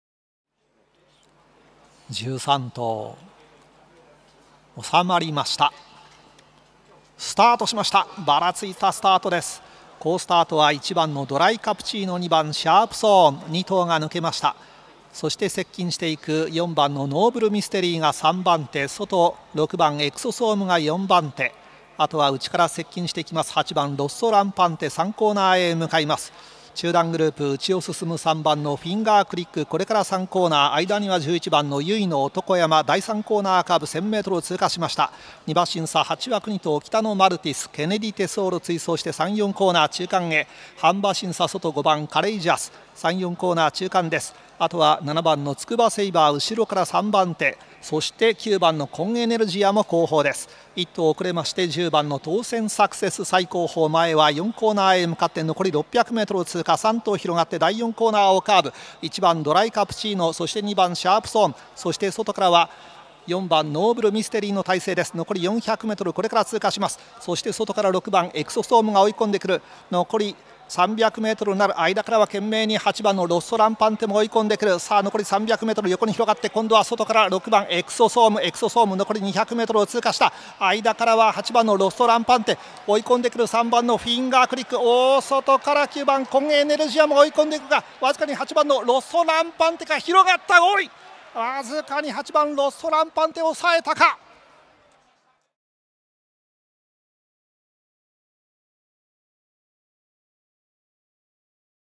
6/11 東京5R 新馬戦 レース映像
6/11 東京5R新馬戦レース映像です！

1着　ロッソランパンテ
2着　フィンガークリック
3着　エクソソーム